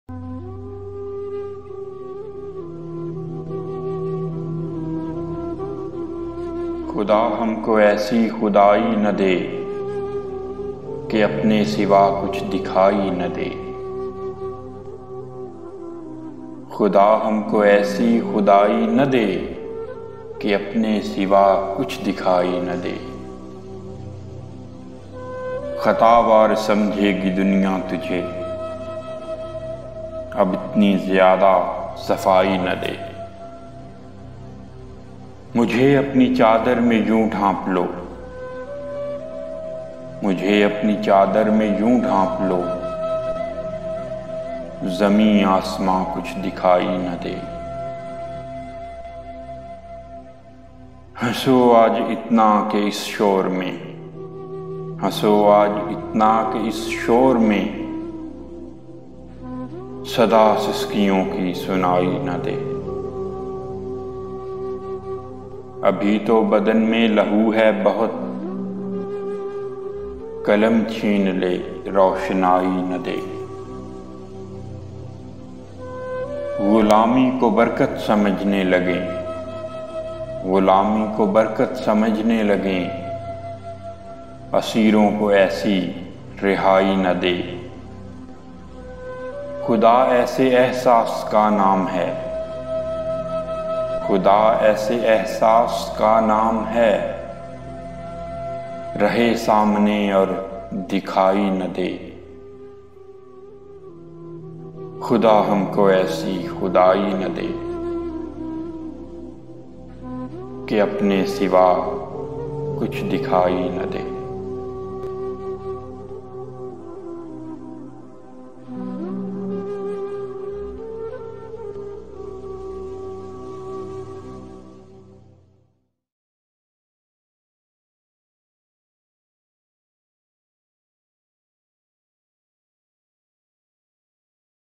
Khuda Hum Ko Aisi Khudaai Na De; Beautiful Poetry of Bashir Badr. Urdu Poetry for WhatsApp Status!
Beautiful Urdu Nazm by Bashir Badr:

خدا ہم کو ایسی خدائی نہ دے
کہ اپنے سوا کچھ دکھائی نہ دے

خطاوار سمجھے گی دنیا تجھے
اب اتنی زیادہ صفائی نہ دے

مجھے اپنی چادر میں یوں ڈھانپ لو
زمیں آسماں کچھ دکھائی نہ دے

ہنسو آج اتنا کہ اس شور میں
صدا سسکیوں کی سنائی نہ دے

ابھی تو بدن میں ، لہو ہے بہت
قلم چھین لے ، روشنائی نہ دے

غلامی کو برکت سمجھنے لگیں
اسیروں کو ایسی رہائی نہ دے

خدا ایسے احساس کا نام ہے
رہے سامنے اور دکھائی نہ دے

khuda ham ko aisi khudai na de
ki apne siva kuchh dikhai na de

khata-var samjhegi duniya tujhe
ab itni ziyada safai na de

hanso aaj itna ki is shor men
sada siskiyon ki sunai na de

ghulami ko barkat samajhne lagen
asiron ko aisi rihai na de

khuda aise ehsas ka naam hai
rahe samne aur dikhai na de